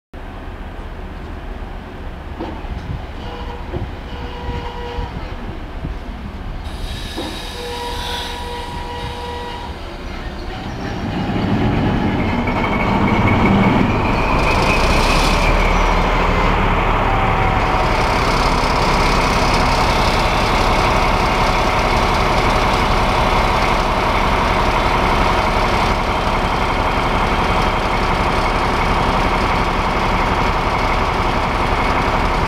Dröhnender Motorstart der Ulmer 218 443 (Motor: TB11 abgasoptimiert)
Motor: 12 V 956 TB 11 abgasoptimiert

Der Motor der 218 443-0 wird in Ulm vor ihrem IRE 4235 nach Lindau Hauptbahnhof gestartet. Eine der letzten verbliebenen Ulmer 218er mit dem alten abgasoptimierten TB11-Motor - und keine davon dröhnt so schön wie die 443!

Aufnahmeort: Ulm Hauptbahnhof, Gleis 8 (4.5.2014)